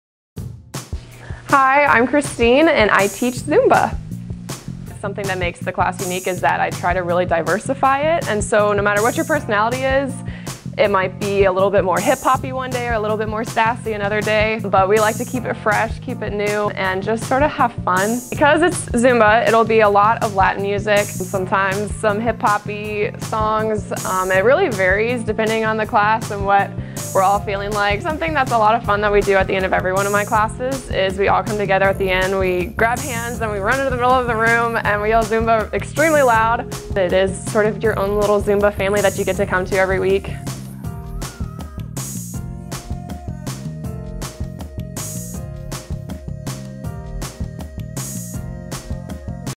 Zumba with Christine
This Zumba class is unique because of the diverse personalities it relates to; weather your style is hip-hop or sass this class will meld with everyone. You can plan on dancing to a lot of latin based music, along with hip-hop depending on the classes mood. Christine likes to keep is fresh, new and fun!

All class pass is available at the member services desk for $55/semester.
To view the full schedule & Instructors bio's visit: go.unl.edu/allclasspass